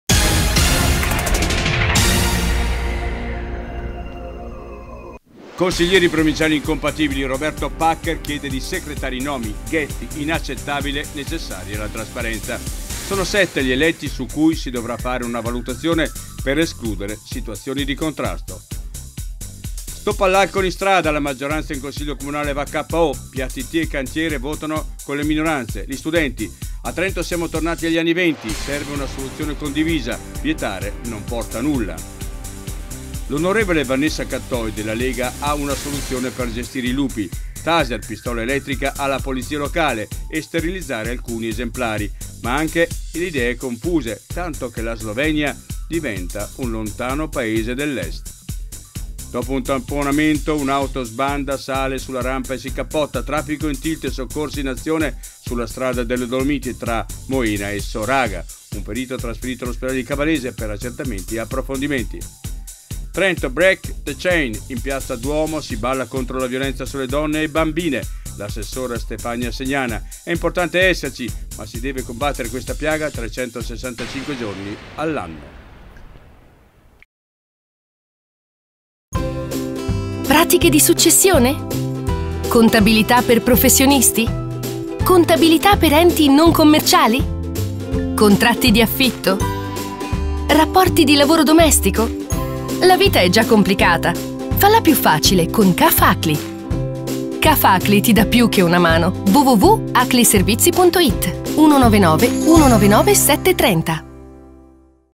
Tg del 14/02/2019
Consiglieri provinciali incompatibili, Paccher chiede di secretare i nomi. Stop all'alcol in strada, maggioranza comunale va ko, gli studenti: "Siamo tornati agli anni '20". L'onorevole Vanessa Cattoi ha la soluzione per gestire i lupi, ma anche le idee confuse. In Val di Fassa un'auto si cappotta. A Trento l'iniziativa "Break the chain" contro la violenza sulle donne e bambine. Le notizie de Il Dolomiti di giovedì 14 febbraio 2019 lette da Nereo Pederzolli con CAF ACLI (Trento) e Radio Italia Anni 60 TrentinoAltoAdige